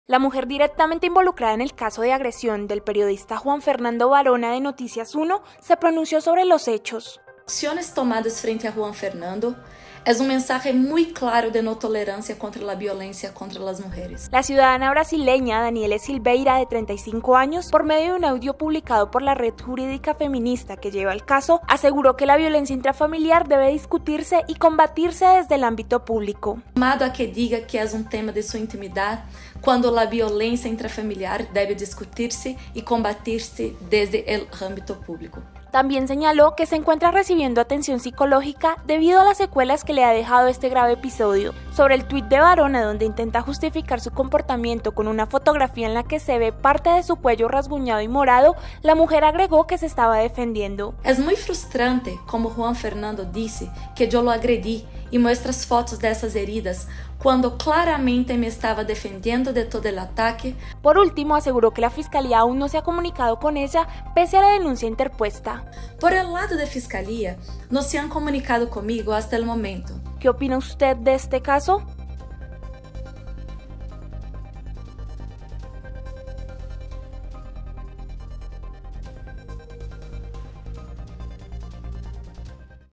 Mujer agredida por periodista se pronunció
La expareja del periodista Juan Fernando Barona, víctima de agresión en un ascensor, se refirió a lo sucedido. Esto fue lo que dijo.

¿Qué opina de este caso?